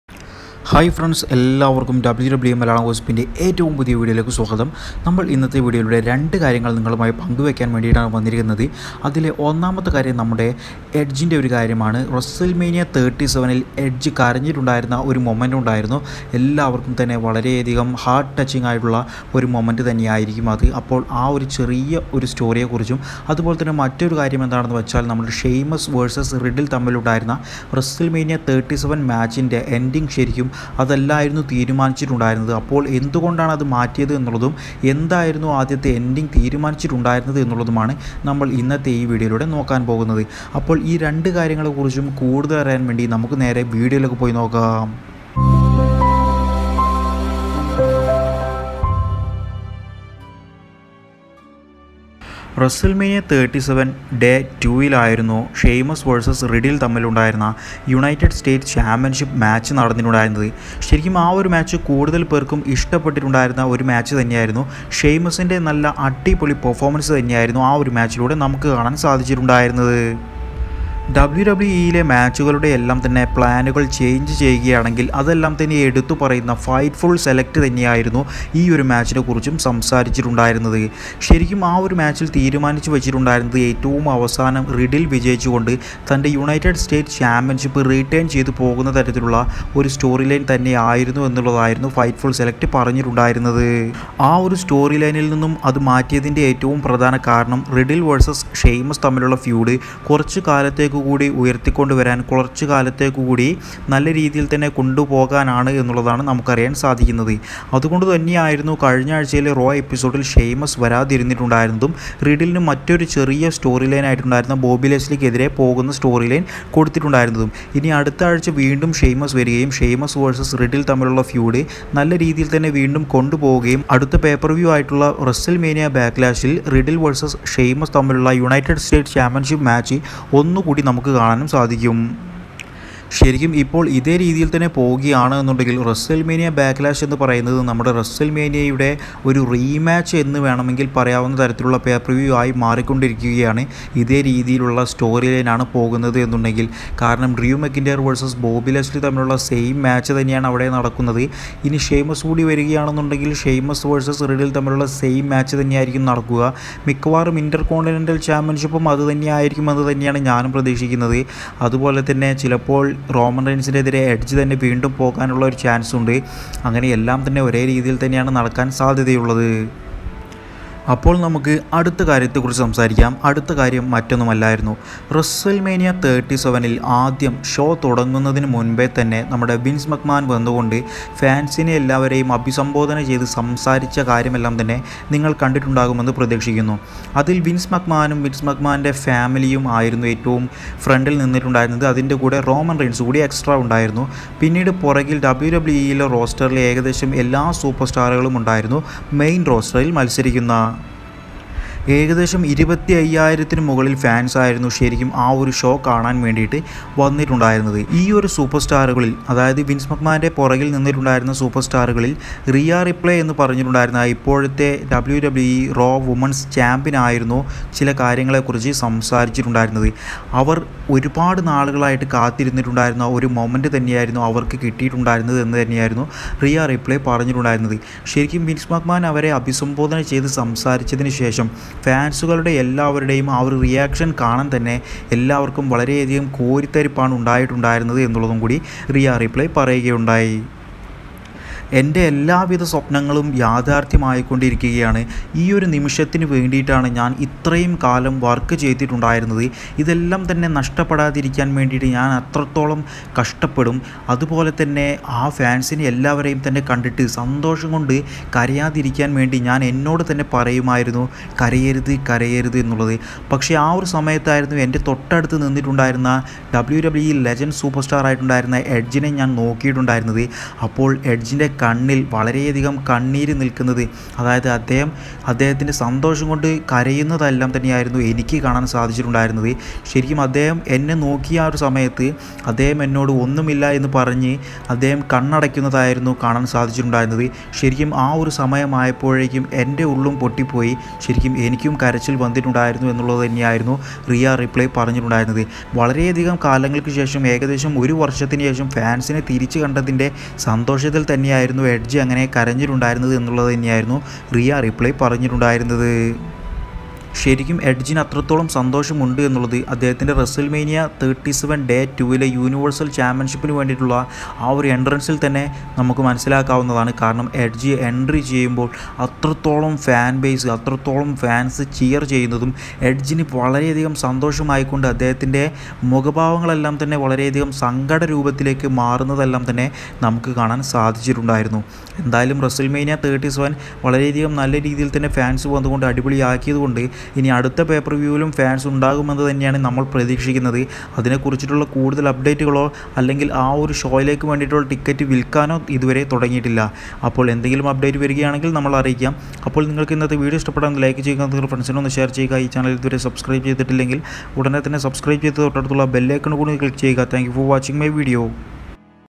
Edge Cried on Wrestlemania 37 😔 | Sheamus not Set to Win US Championship 😲 | WWE Malayalam
Edge Cried on Wrestlemania 37 | Sheamus not Set to Win US Championship